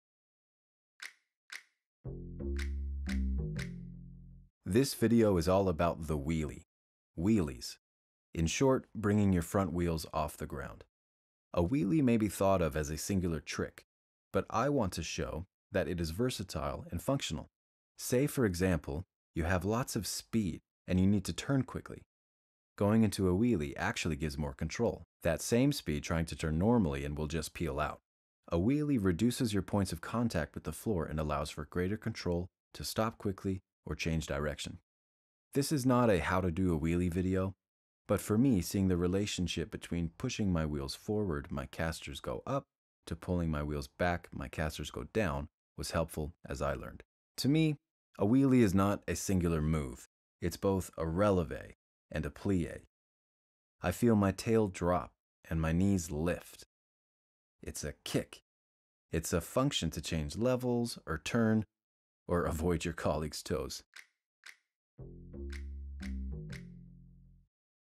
Wheelie - Tips and Tricks
To me, a wheelie is both a relevè and a pliè. It serves many purposes.

Credits:
-Joel Brown- writer, dancer, composer
-George Bushaway- director/editor
-Zoe Fayaud- filming

These videos were made possible by  Unlimited.